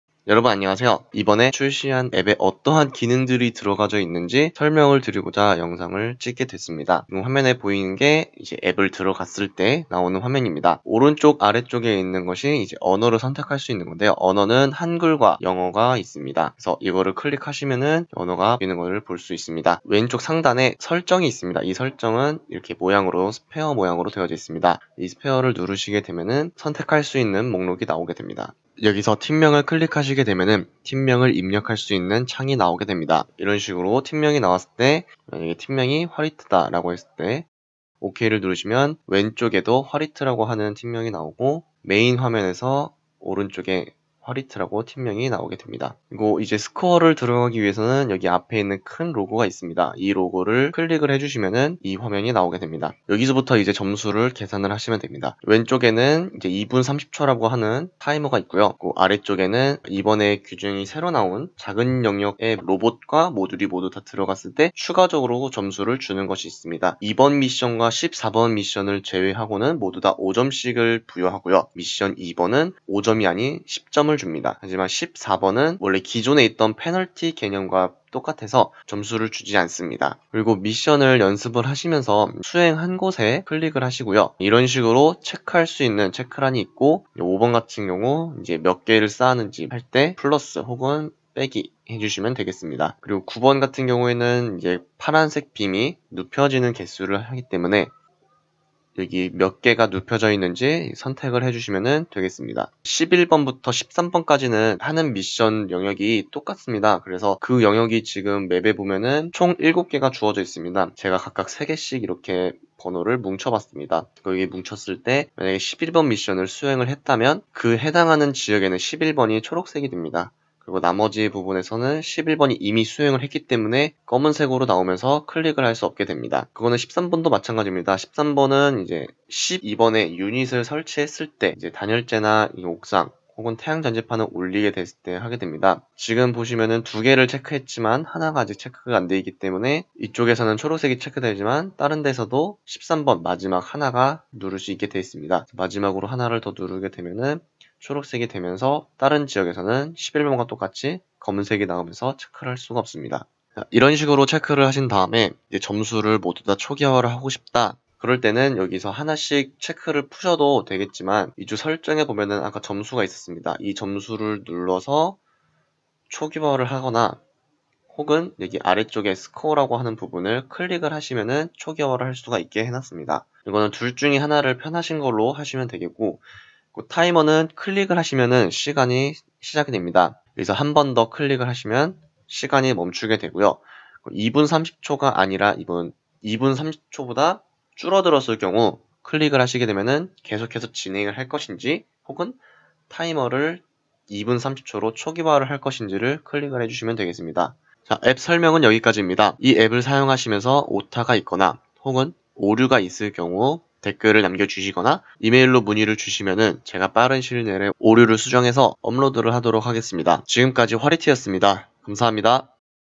2019-2020 FLL CITY SHAPER Scorer 어플 설명 - 화리트(Hwarite)
이번 영상은 FLL 대회 점수 계산 어플입니다. 채점지와 다르게 경기장 사진에 나와있는 각 미션의 수행을 체크하는 어플입니다.
한글과 영어를 포함되어 있어서 편리한 언어로 선택을 해주시면 되겠습니다.

앱은 구글 스토어에서만 지원이 됩니다.